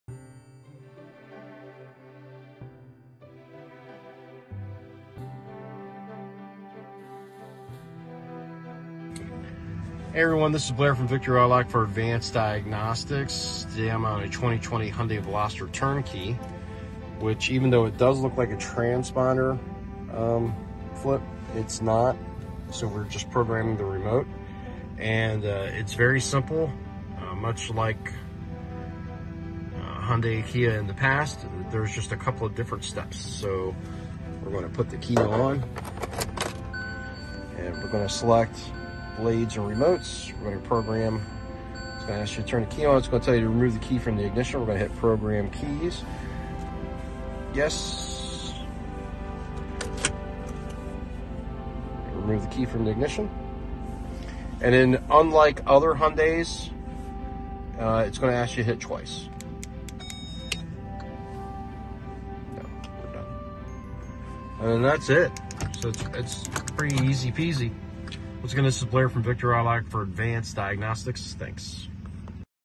2020 Hyundai Veloster key-less entry programming
2020 Hyundai Veloster Keyless entry programming! Rob Blair Boothe demonstrates how elementary programming can be.

Keyless entry programming is token free & if appropriately priced, very lucrative. The sticky point for some security professionals is choosing the incorrect part or accepting customer-purchased products from an unknown source. Therefore charge for your time, only work for a fee.